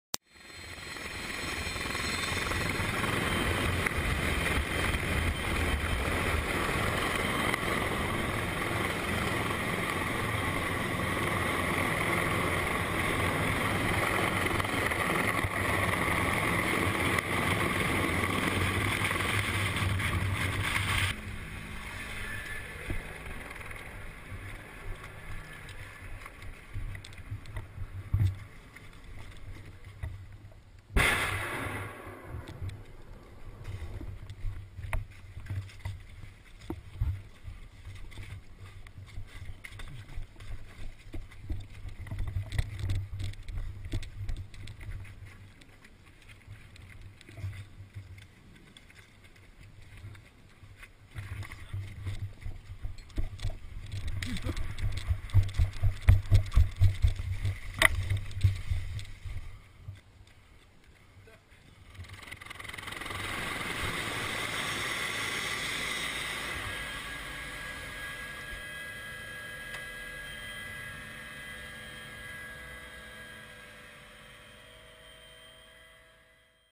Norwegian Special Forces Helmet Cam
helmet cam view of a Norwegian Special Forces exercise.